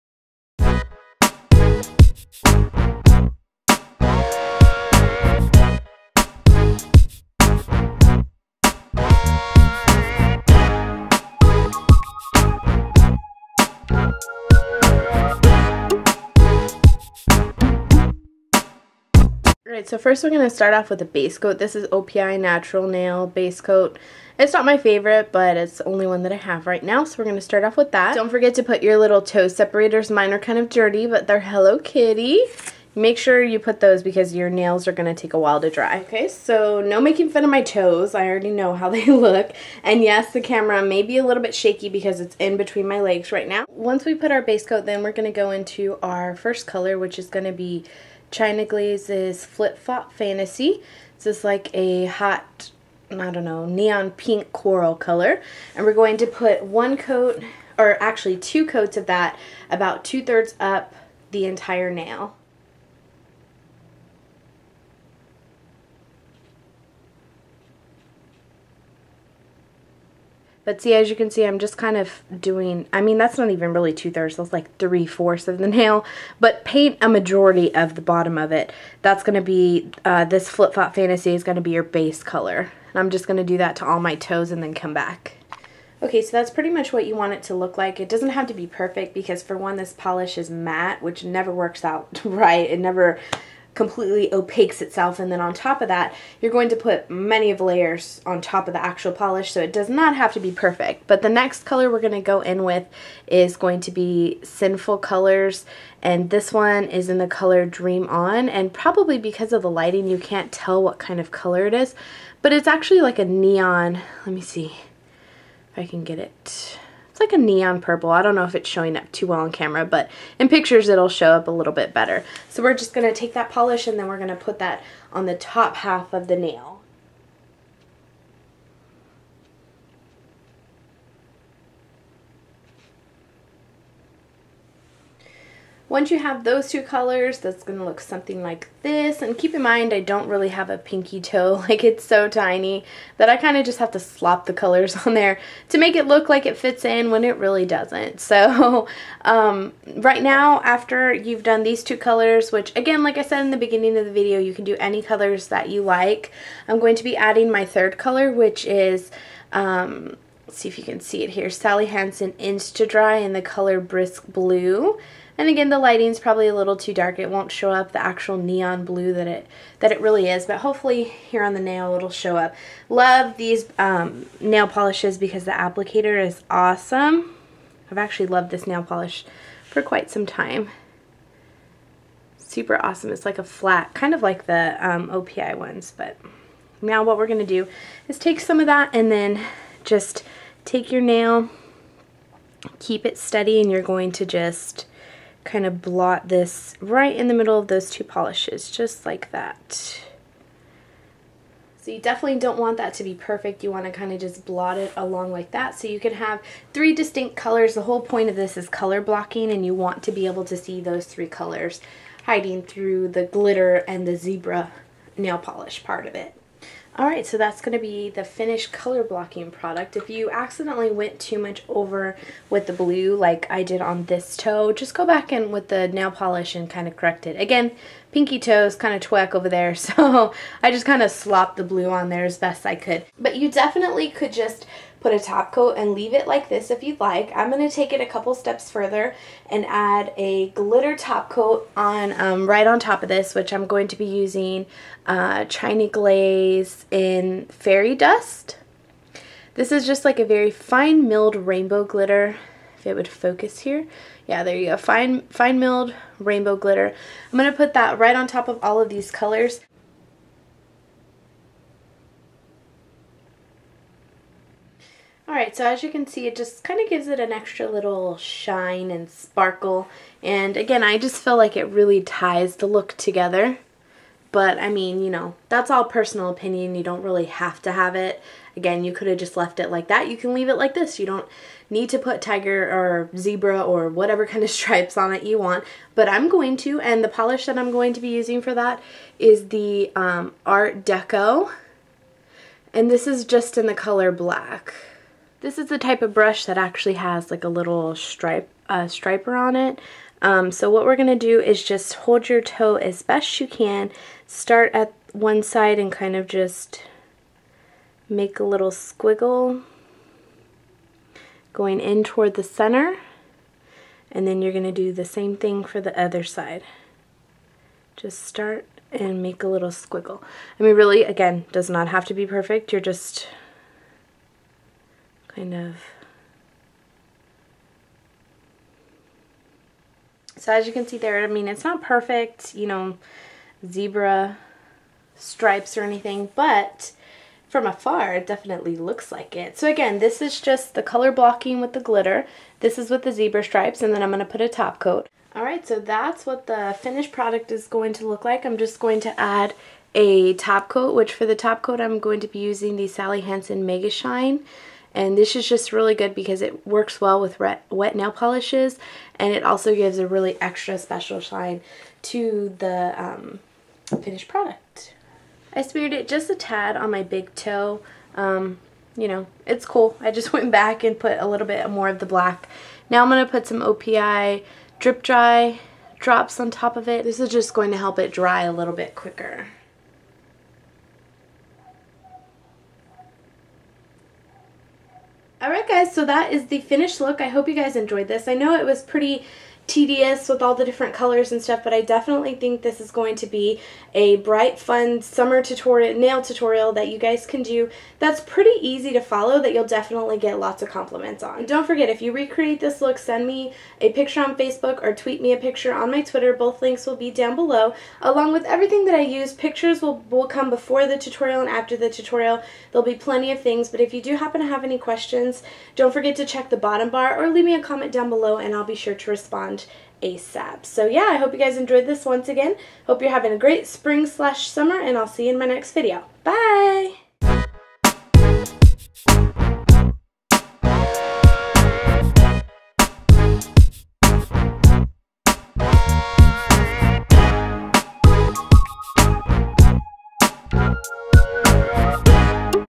Color Blocking, Glittery, Zebra Fun Summer Toenail Tutorial!
I haven't done a Pedicure Tutorial in such a long time! I really hope you guys enjoy! I will link the other ones I've done down below.

xoxo Melissa

Oh and just a side note...this pedicure was recorded almost 2 weeks ago and it is still going strong with absolutely no chips!!

Products Used:
OPI Natural Base Coat
China Glaze Flip Flop Fantasy
Sinful Colors Dream On
Sally Hansen InstaDry Brisk Blue
China Glaze Fairy Dust
Art Deco Nail Striper Black
Sally Hansen Mega Shine Top Coat
OPI Drip Dry Drops